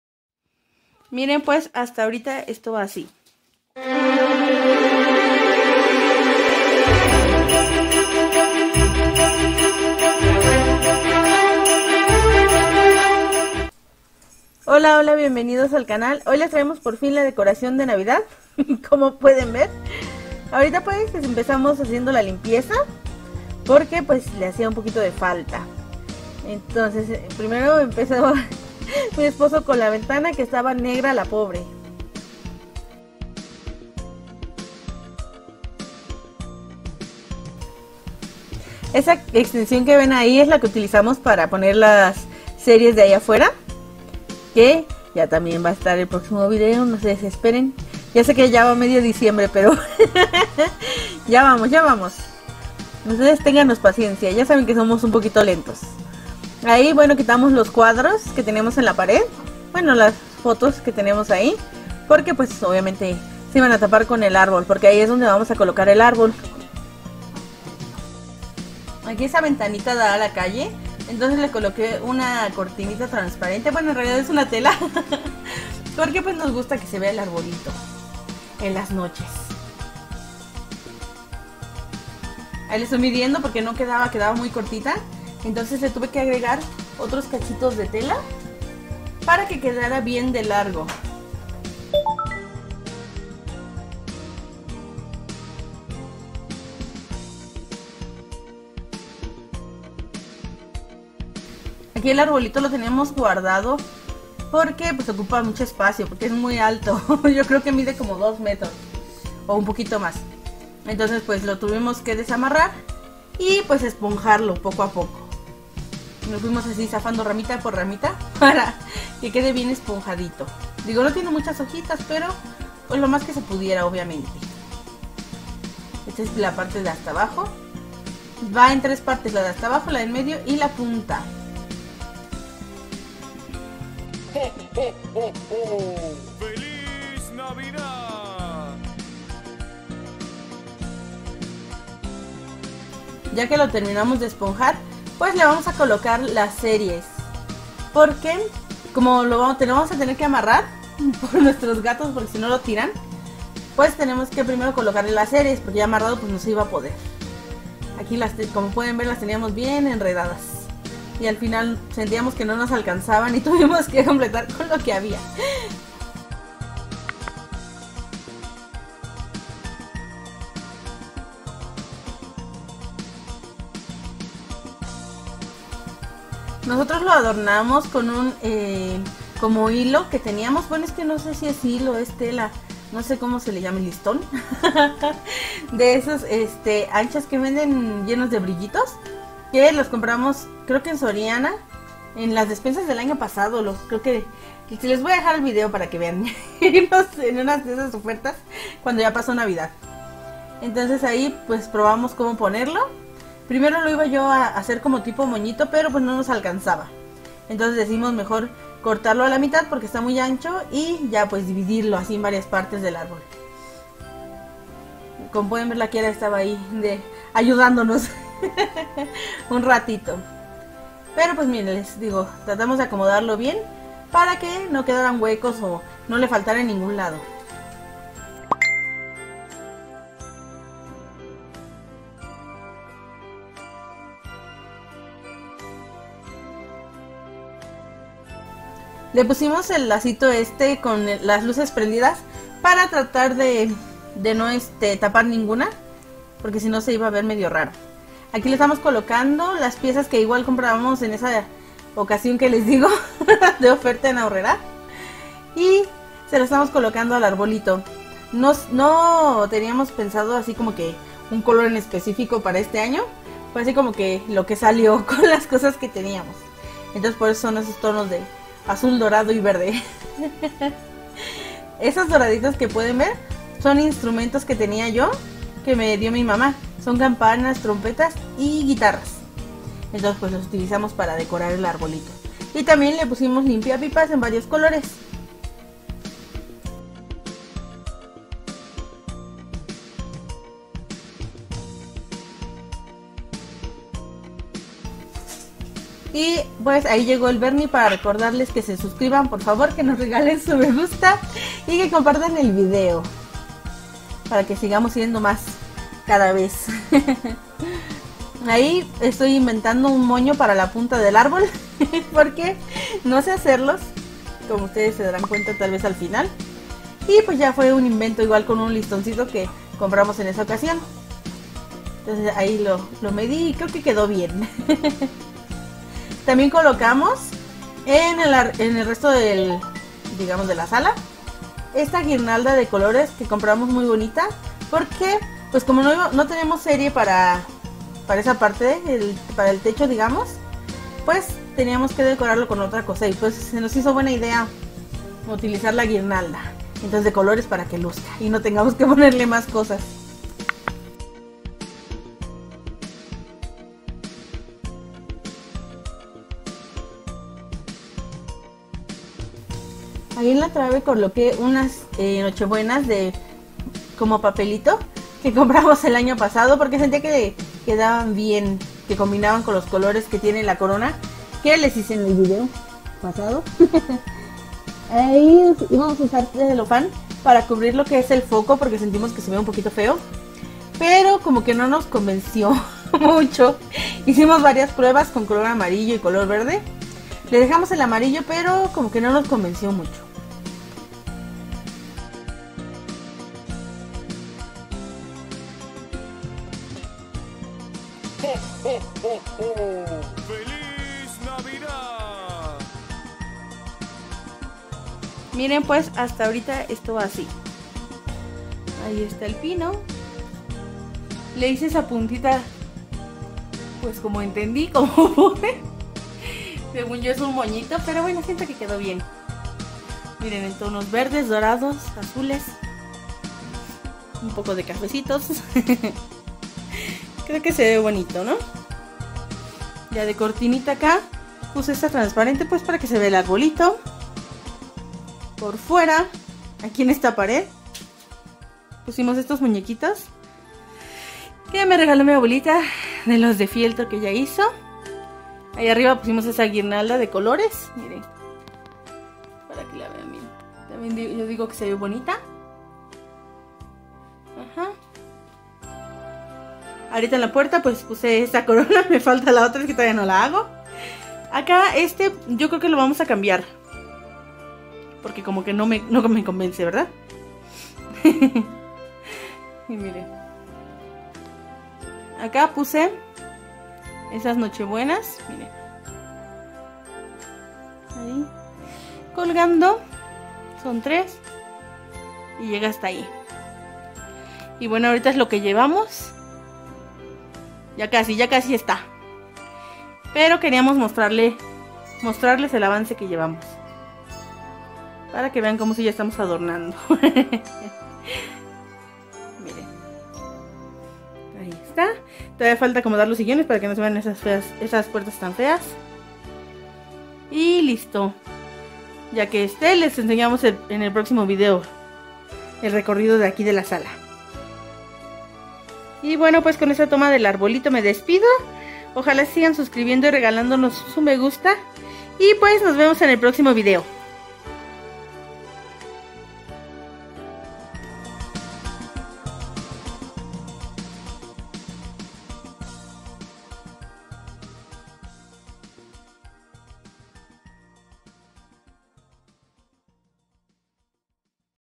mi árbol de NAVIDAD | Leslie Viri
El árbol de Navidad es un elemento decorativo de gran simbolismo para la celebración de la Navidad. ... Su color verde y su forma de punta son símbolos de la vida y el amor de Dios, y las luces que lo adornan nos remiten al misterio de la Nochebuena, cuando Jesucristo llega al mundo trayendo la luz de una nueva esperanza

¿Cuándo se debe colocar el árbol de navidad?

De acuerdo a lo estipulado a la religión católica, la fecha correcta para colocar y adornar el árbol es el primero de diciembre, cuando da inicio el Adviento, un periodo de preparación para celebrar la Navidad, el cual comienza cuatro domingos antes de esta celebración

El árbol de Navidad o árbol navideño es un elemento decorativo, típico de la fiesta de Navidad.

Los elementos decorativos del árbol de Navidad que nosotros consideramos debe llevar son:

Estrella o moño: colocado generalmente en la punta del árbol, representa la fe que debe guiar la vida del cristiano, recordando a la estrella de Belén.
Esfera:  se acostumbra a colocar bolas o esferas (en algunas regiones también llamadas bambalinas o chirimbolos), que simbolizan los dones de Dios a los hombres.
Lazos: Siempre se ha pensado que los lazos representan la unión de las familias y personas queridas alrededor de dones que se desean dar y recibir.
Luces: en un principio velas, representan la luz de Cristo.

En nuestro caso elegimos los siguientes colores:

El azul para las oraciones de reconciliación.
El plata para las de agradecimiento.
El oro para las de alabanza.
El verde para las de abundancia, fortaleza y naturaleza.